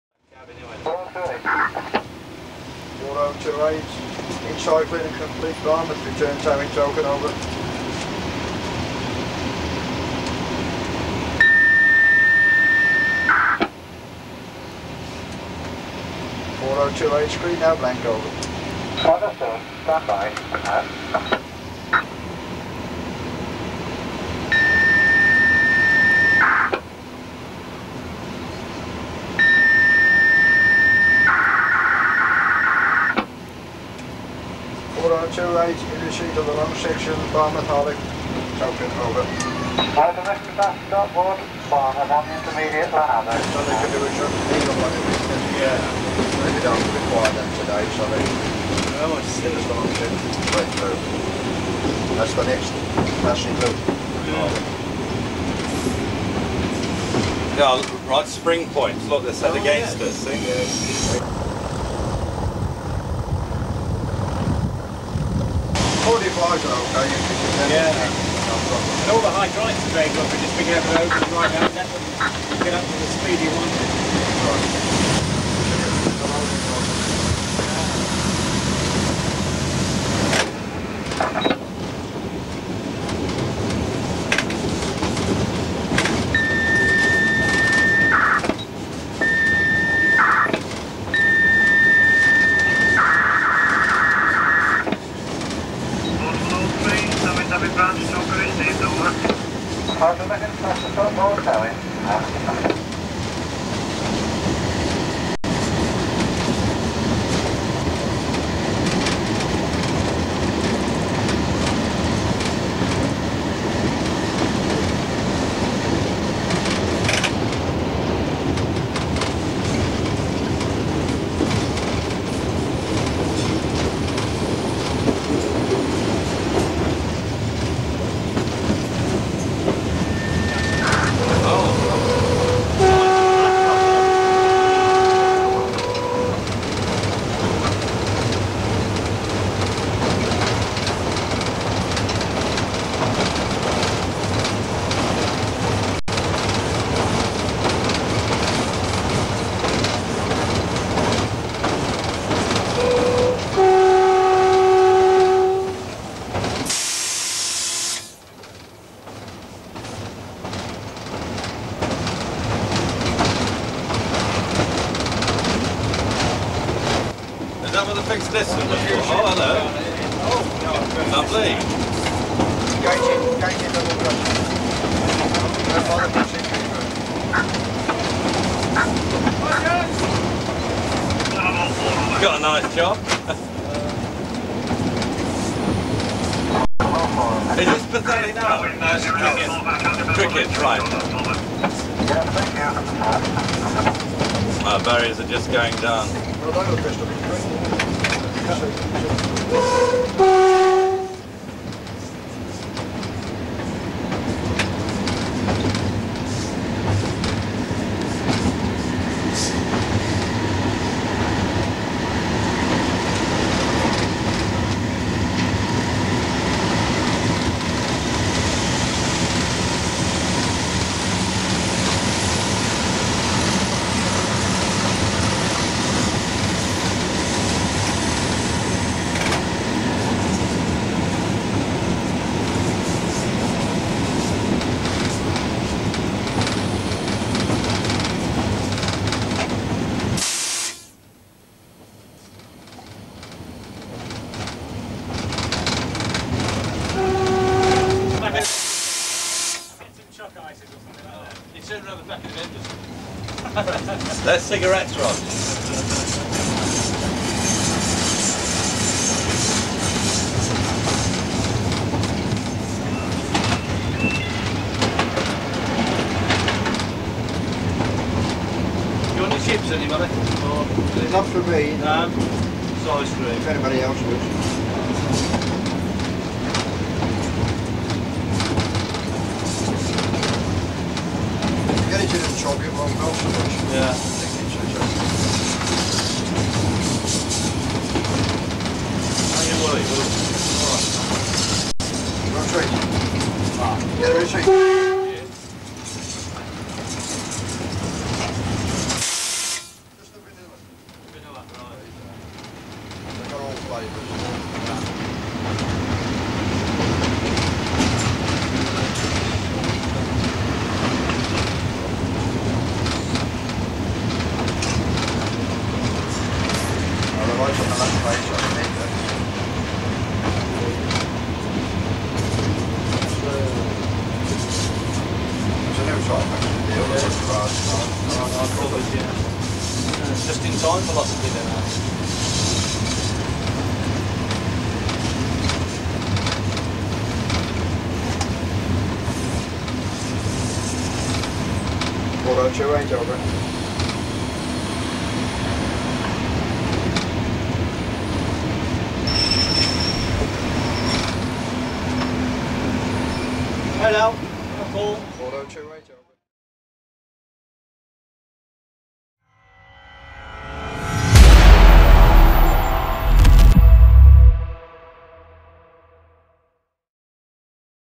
Cambrian Coast Cab Ride Part 2 (1992)
Continuing along the Cambrian coast to the end of the line at Pwllheli, we reboard the Class 31 loco and witness the operation of RETB signalling from the perspective of the train crew. This method of signalling lasted on the Cambrian until March 2011 when the whole route adopted the ERTMS method of signalling. Plans are in progress to further enhance the system with the use of ETCS which will enable in-cab digital signalling  which is used now widely around Europe with new installations.